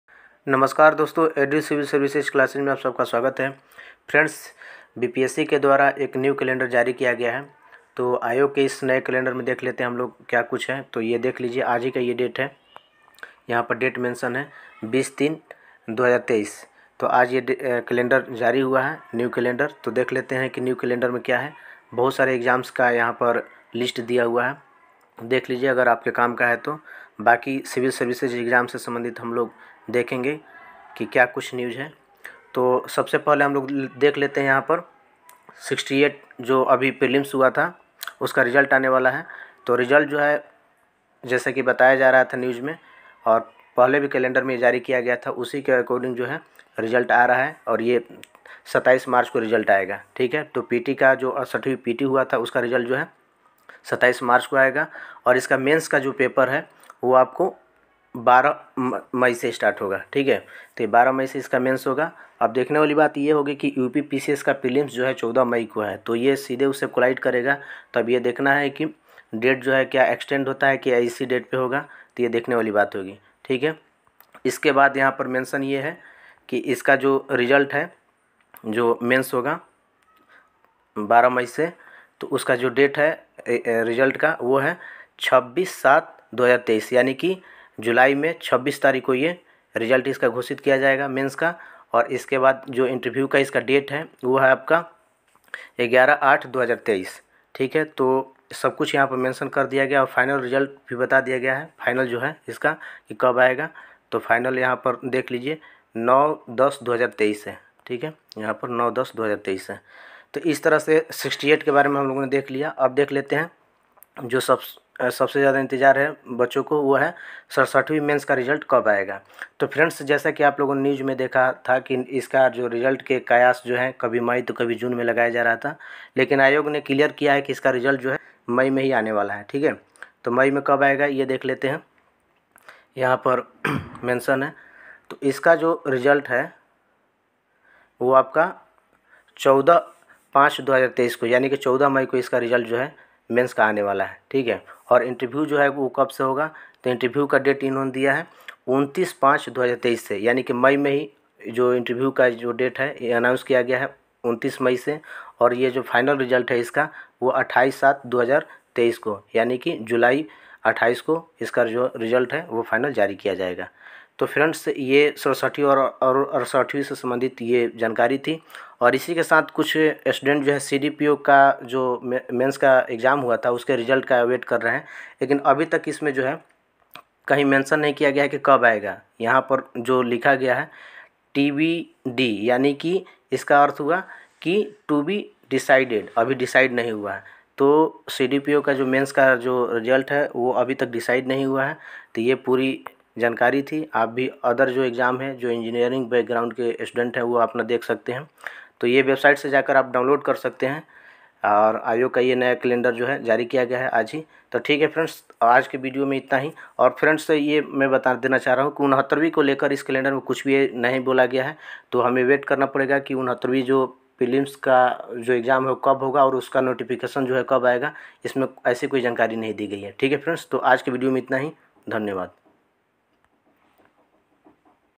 BPSC ने जारी किया नया एग्जाम कैलेंडर,67 BPSC mains result?69 के exam date पर क्या क्या है update
BPSC NEW exam calendar bpsc latest news#67th bpsc mains result 68th bpsc prelims result#69th bpsc notification bihar news#ad_civil_services